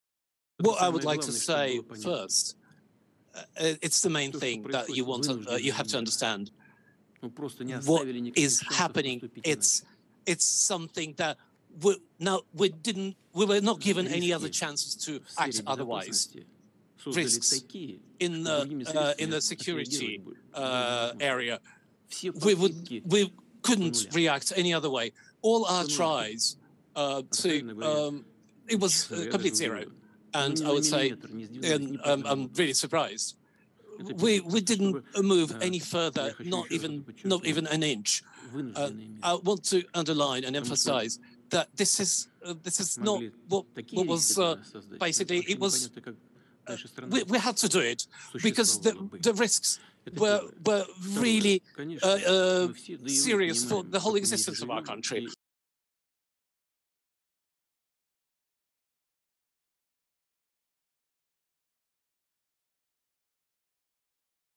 Putin claims Russia 'was given no other choice' but to invade Ukraine
Vladimir Putin has warned the West it will face "untold consequences" if challenged as he launched the long-feared invasion in Ukraine.

The Russian President also vowed to "demilitarize" and "de-Nazify" Ukraine in a sinister televised speech.

Putin stated that "modern Russia, even after the collapse of the USSR and the loss of a significant part of its nuclear potential, is today one of the most powerful nuclear powers."

And in a chilling warning to the West he added: "To anyone who would consider interfering from outside: If you do, you will face consequences greater than any you have faced in history."